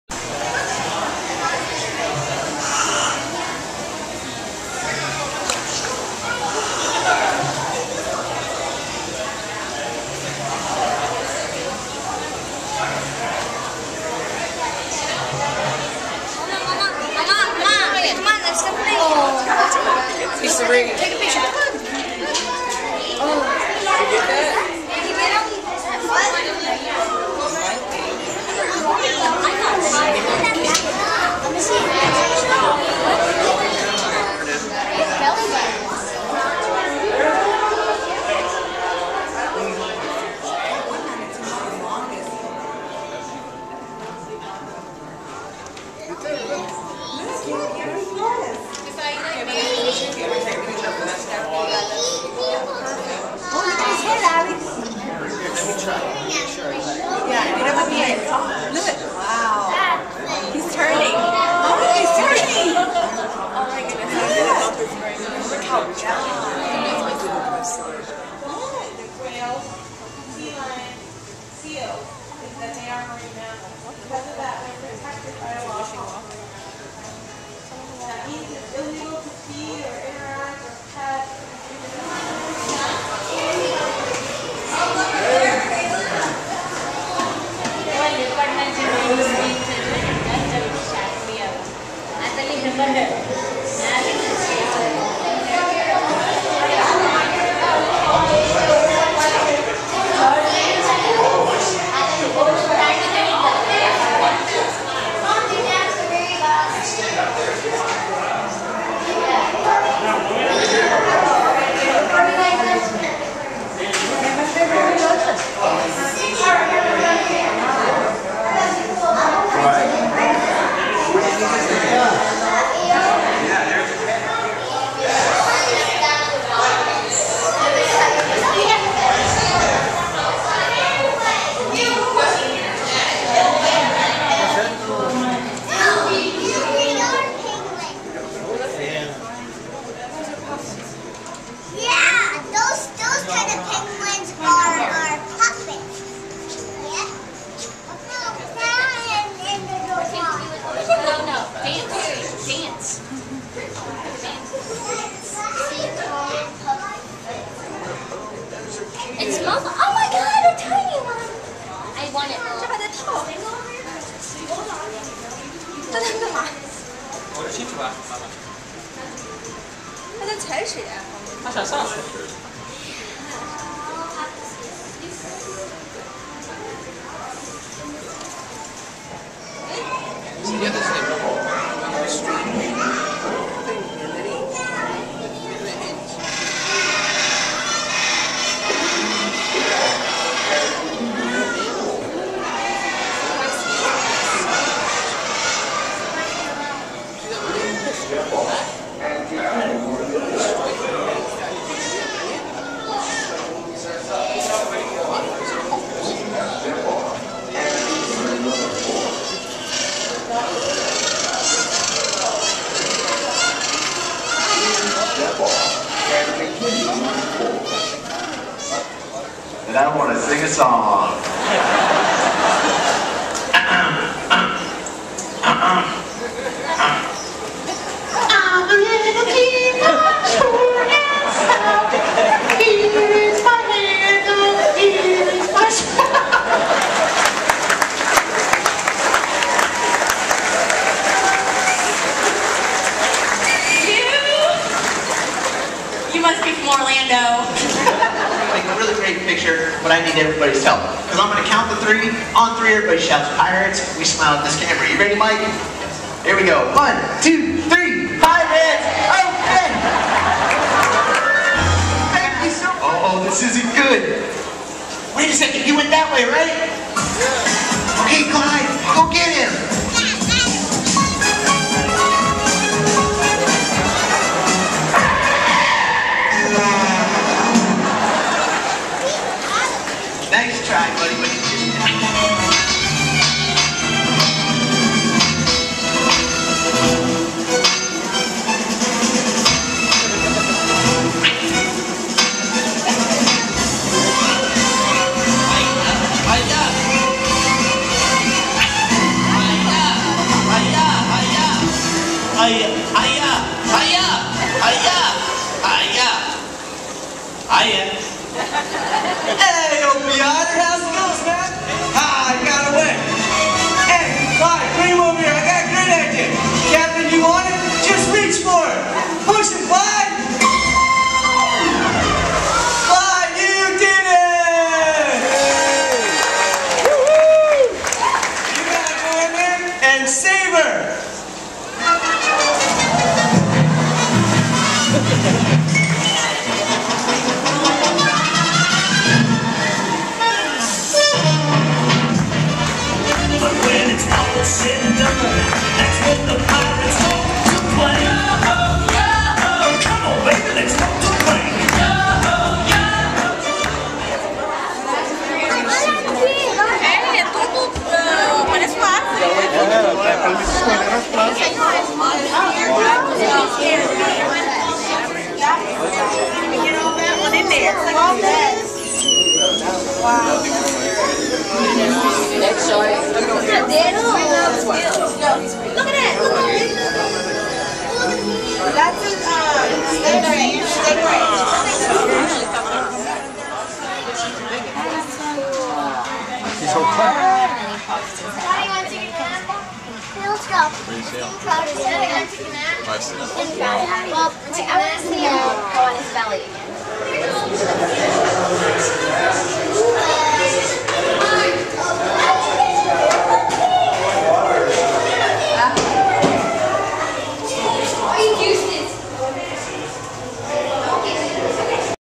12 - NOT Disney World - Part 1 (Sea World)
For a couple days we get away from the Disney stuff and go to other parks in the area. First we start off with Sea World. ... In the video you'll see a lot of sea life including manta rays, dolphins, manatees, penguins, sharks, and a walrus.

It was very very hot that day, and sitting in the sun waiting for Shamu would not be fun for us. So we decided to skip that. ... We did however see a very funny show involving pirates with a sea lion and an otter. I think they were having some audio issues, but still had a lot of fun because the actors kept cracking up. In one of the videos you see the guy on the boat going "high-ya!" and then seem to signal someone about the audio ... like "Ok you can stop now." lol ... Apparently their mics were going on and off and had some cheesy back up recorded audio played at points.

Tomorrow (our next video) we visit Universal Studios! ... And then I think it'll be a 2 video day, because we also visited Disney Downtown the same day. *** BE FRIENDS with JOEL RYAN on FACEBOOK